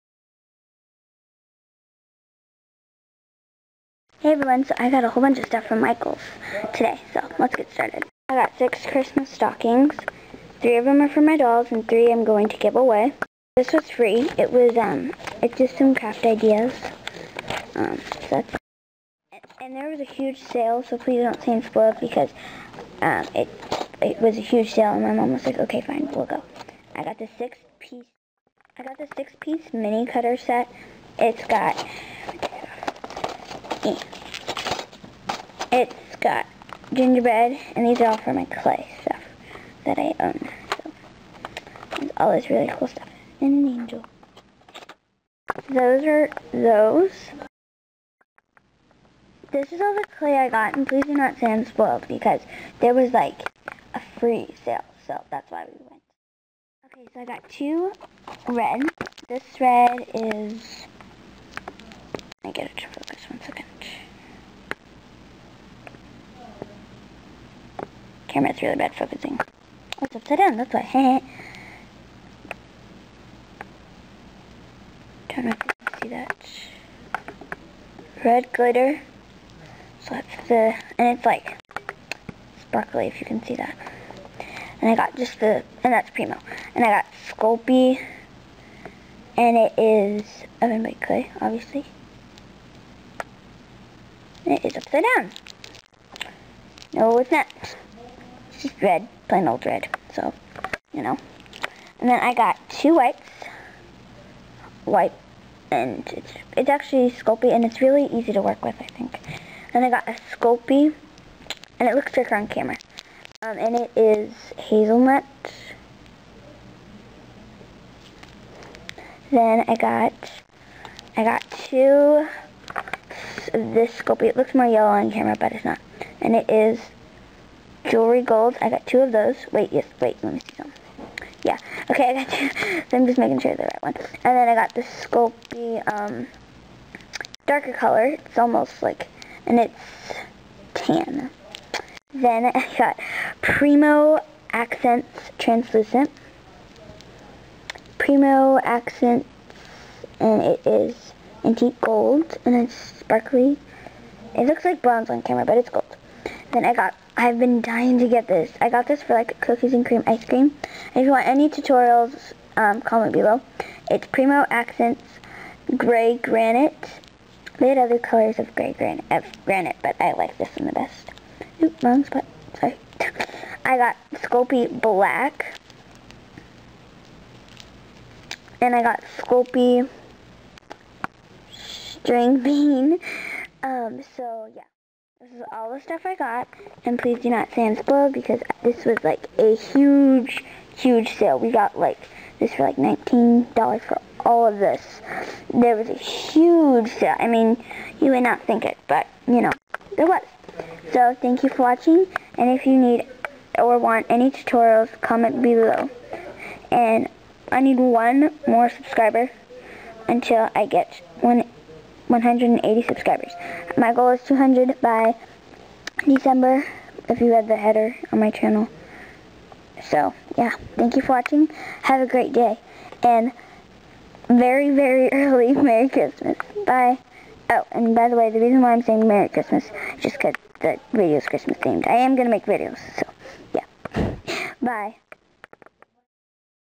What I got at a craft store
PLELASE DONT SAY I AM SPOILED IT IS MY THANKSGIVING PRESENT SO DONT PLEASE IF YOU DO I WILL BLOCK YOU!
I love everything I got and I will have a stopmotion up soon